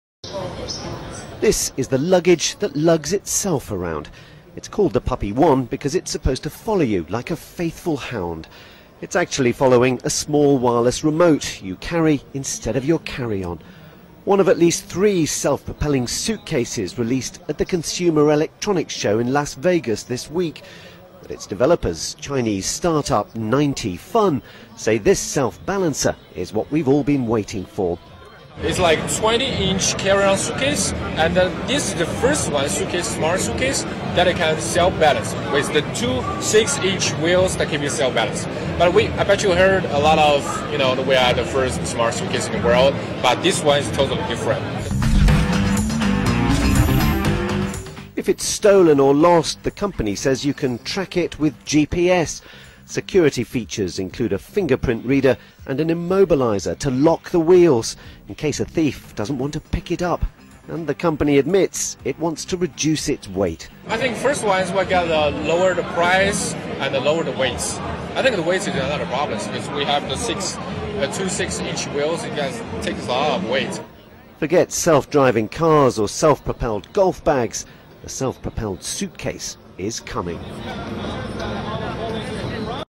Self-balancing smart suitcase follows you around/Youtube News
The latest so-called smart suitcase, that's a suitcase that follows you around, has only two wheels and auto-balancing technology and a price yet to be announced. Stuart McDill reports. Video provided by Reuters Newslook